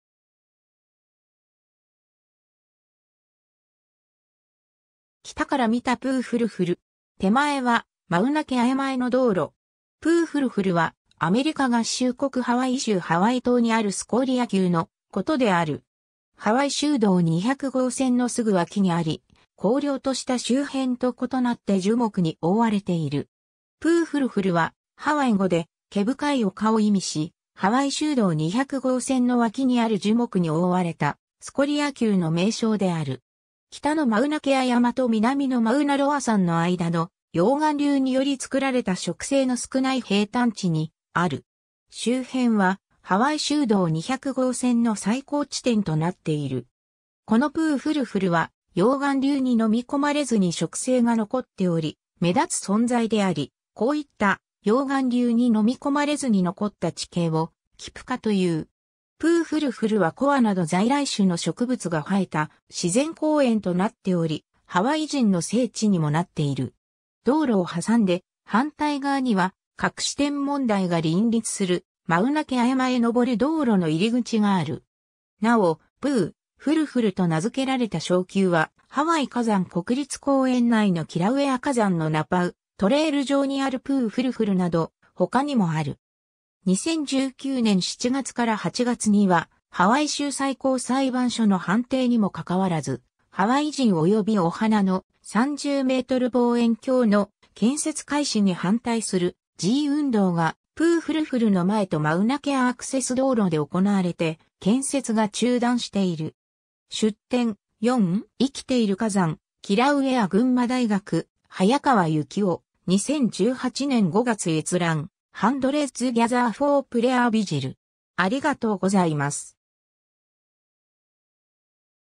プウ・フルフル (ハワイ州道200号線)
北から見たプウ・フルフル。
手前はマウナ・ケア山への道路。
 プウ・フルフル（英語: Pu'u Huluhulu）は、アメリカ合衆国ハワイ州ハワイ島にあるスコリア丘のことである。
ハワイ州道200号線のすぐ脇にあり、荒涼とした周辺と異なって樹木に覆われている。
 プウ・フルフルはハワイ語で「毛深い丘」を意味し、ハワイ州道200号線（旧称：サドルロード Saddle Road）の脇にある樹木に覆われたスコリア丘の名称である。
北のマウナ・ケア山と南のマウナ・ロア山の間の、溶岩流によりつくられた植生の少ない平坦地にある。
周辺はハワイ州道200号線の最高地点（2,021m）となっている。
 このプウ・フルフルは溶岩流に飲み込まれずに植生が残っており目立つ存在であり、こういった溶岩流に飲み込まれずに残った地形をキプカ（ハワイ語が語源）という。
 プウ・フルフルはコアなど在来種の植物が生えた自然公園となっており、ハワイ人の聖地にもなっている。
道路を挟んで反対側には各種天文台が林立するマウナケア山へ登る道路の入口がある。
 なお、プウ・フルフルと名づけられた小丘は、ハワイ火山国立公園内のキラウエア火山のナパウ・トレイル（Napau Trail）上にあるプウ・フルフルなど、他にもある。
 2019年7月～8月には、ハワイ州最高裁判所の判定にもかかわらず、ハワイ人およびオハナの「30メートル望遠鏡」（TMT）の建設開始に反対する示威運動が、プウ・フルフルの前とマウナケア・アクセス道路で行われて、建設が中断している。
 ^ 出典(同名について) : 4．生きている火山キラウエア（ハワイ島の火山見学案内、1997年） - 群馬大学 早川由紀夫、2018年5月閲覧 ^ Hundreds gather for prayer vigil (Hawaii Tribune-Herald, 2019)